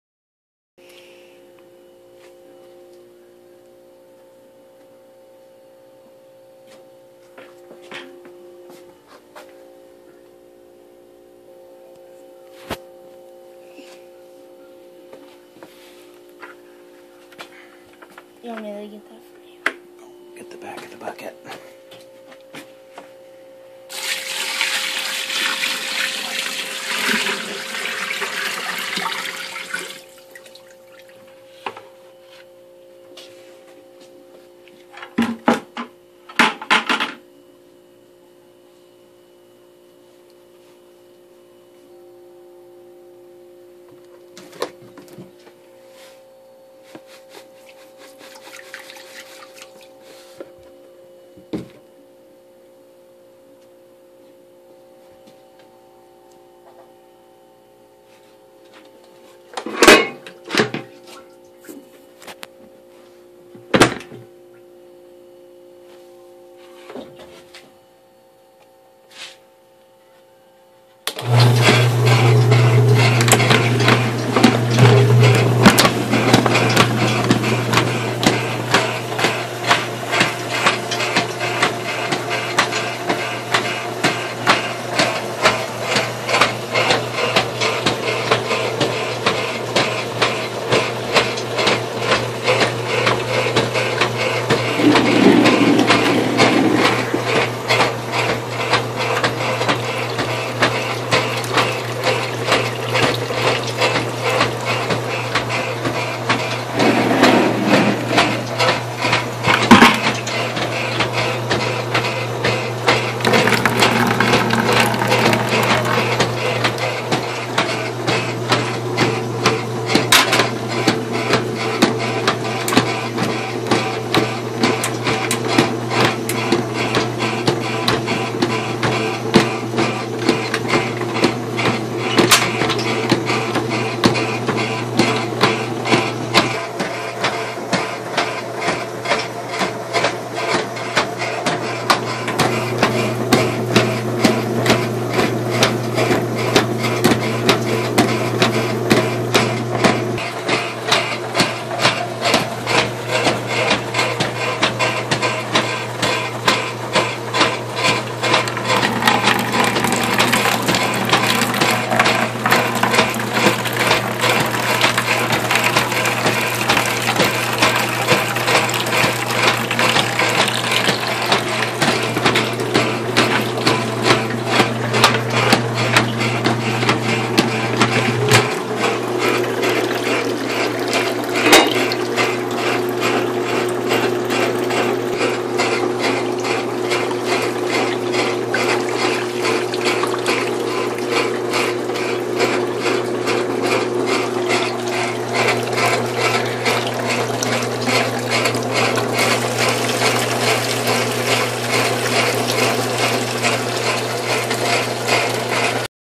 Copper Washing Machine "Automatic Electric Washer Co" Newton Iowa
I use this to wash my shop rags, This machine will outlast any washing machines built today. I had to empty and rinse 3 times, I guessed on the soap each time.